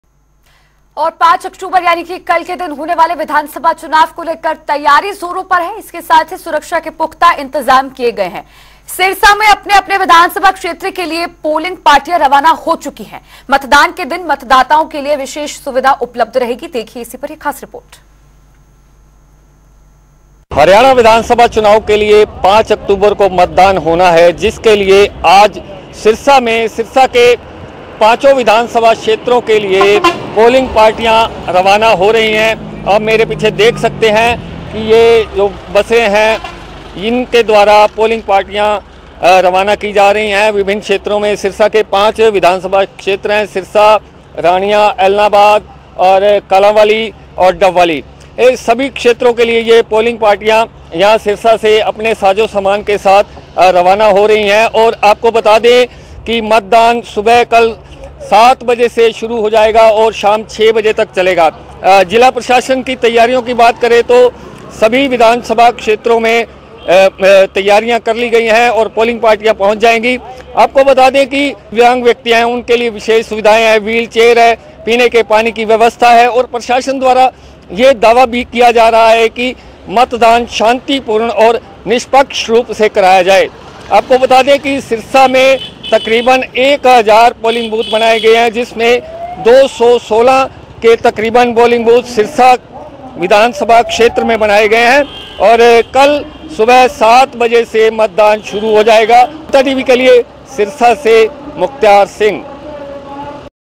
Sirsa विधानसभा क्षेत्र के लिए पोलिंग पार्टियां चुनाव सामग्री लेकर रवाना, प्रशासन की कैसी तैयारी...
Sirsa विधानसभा क्षेत्र के लिए पोलिंग पार्टियां चुनाव सामग्री लेकर रवाना, प्रशासन की कैसी है तैयारियां, जानिए...